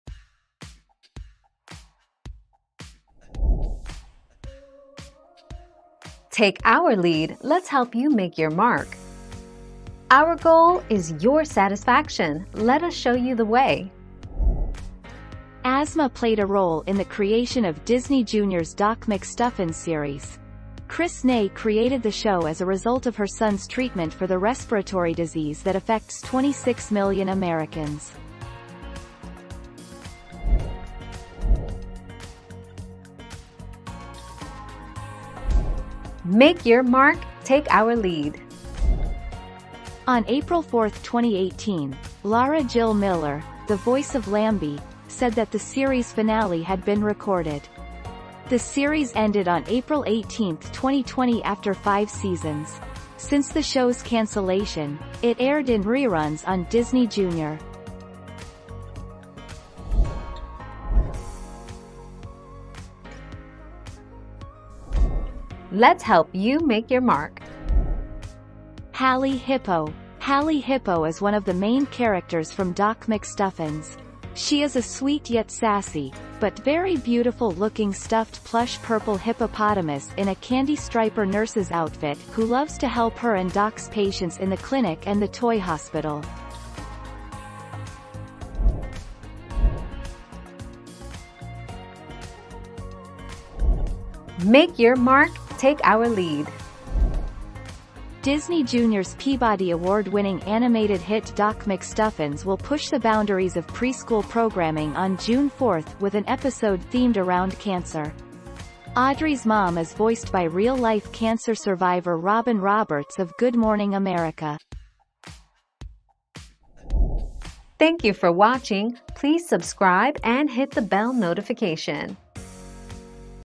What disease does Doc McStuffins have?
00:00 - What disease does Doc McStuffins have?
00:32 - Did Doc McStuffins get Cancelled?
01:01 - Who is the hippo on Doc McStuffins?
01:30 - Is Doc McStuffins a little girl who has cancer?

Laura S. Harris (2021, June 4.) What disease does Doc McStuffins have?
    AskAbout.video/articles/What-disease-does-Doc-McStuffins-have-250991